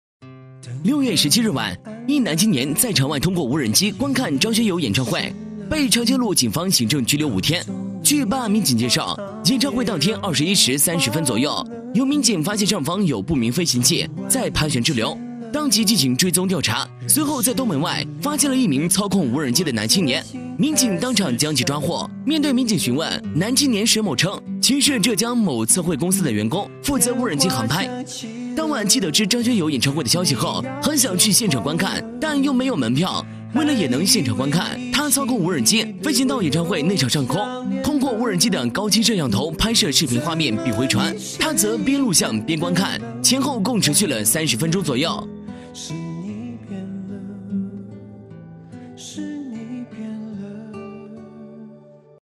【3年前】男青年用無人機看張學友演唱會 被民警發現拘留5天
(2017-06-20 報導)

6月17日晚，一男青年在場外通過無人機觀看張學友演唱會，被長興路警方行政拘留5天。據辦案民警介紹，演唱會當晚21時30分左右，有民警發現上空有不明飛行器在盤旋滯留，當即進行跟蹤、調查。隨后在東門外發現了一名操控無人機的男青年，民警當場將該其抓獲。